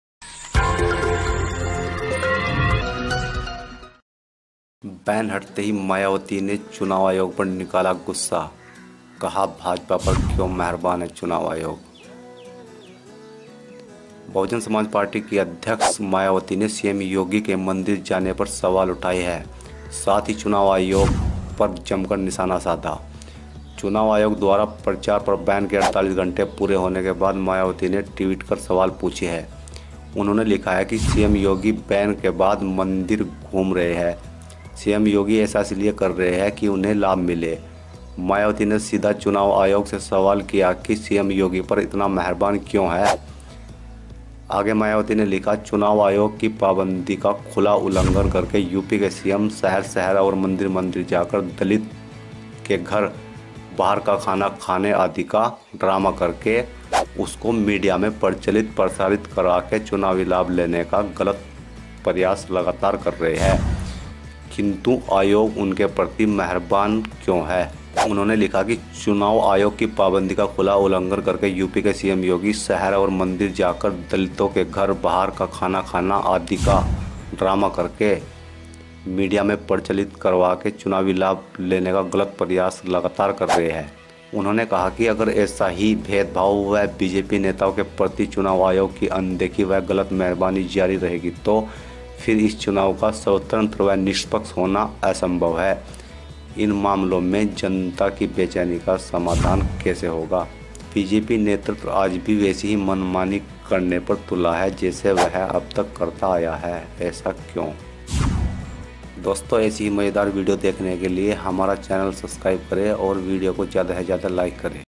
बैन हटते ही Mayawati ने चुनाव आयोग पर निकाला गुस्सा, CM Yogi पर साधा निशाना | Dainik Jagat
Mayawati withdraws ban on Election Commission, anger on CM Yogi

Mayawati announces withdrawal of ban on Election Commission